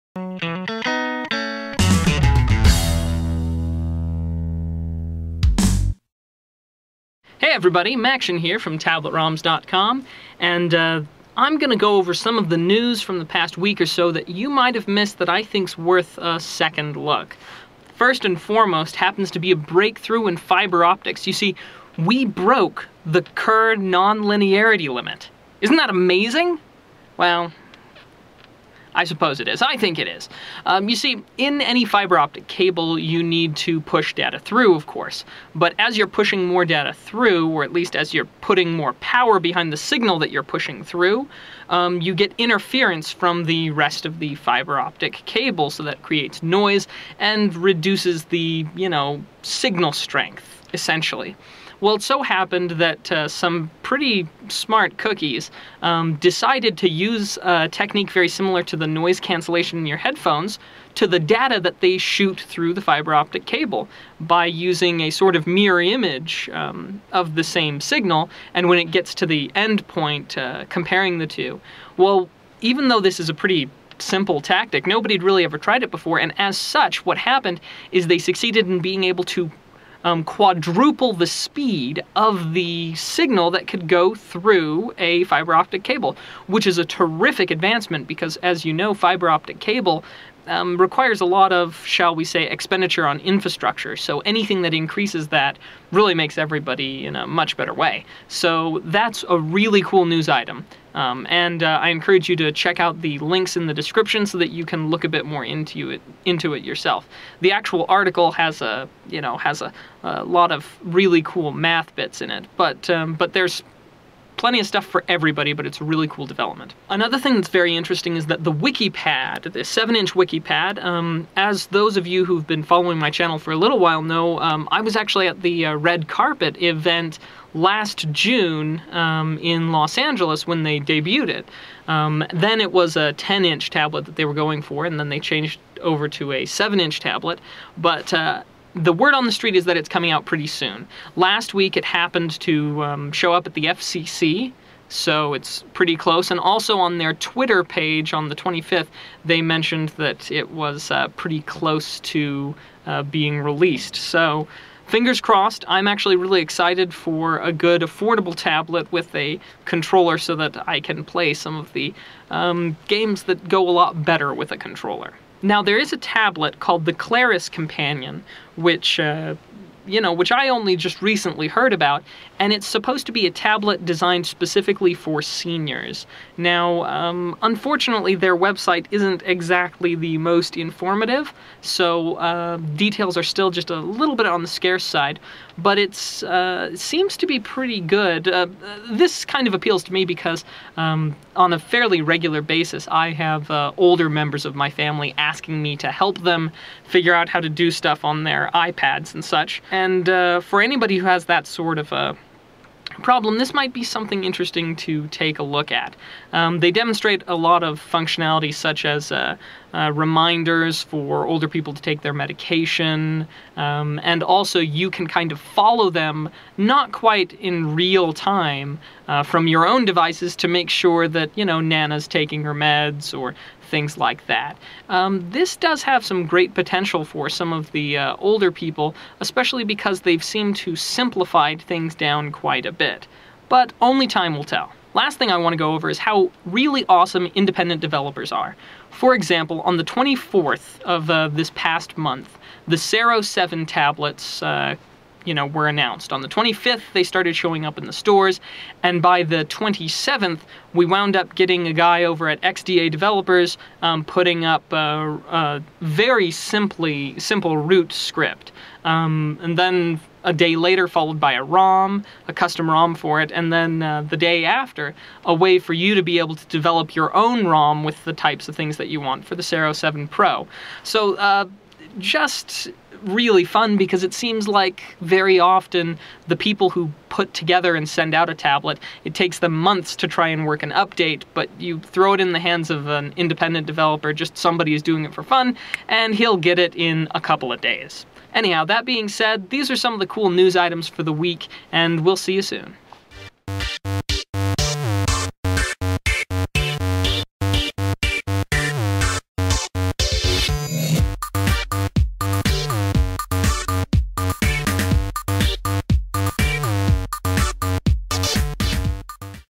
News 2013 06 02
Some of the more interesting developments in the news for Tablets, and technology in general... this week.

FiberOptics!  We broke the Kerr nonlinearity limit!

Wikipad comes out soon?
Yes it does, Release date: June 11th.

Finally, a tablet, other than the iPad, for seniors!

Developers are awesome!
Sero 7 tablets come out on the 24th, and in 3 days one guy has made super easy Root scripts, custom ROMs, and a good way to build your own custom ROM for the tablet.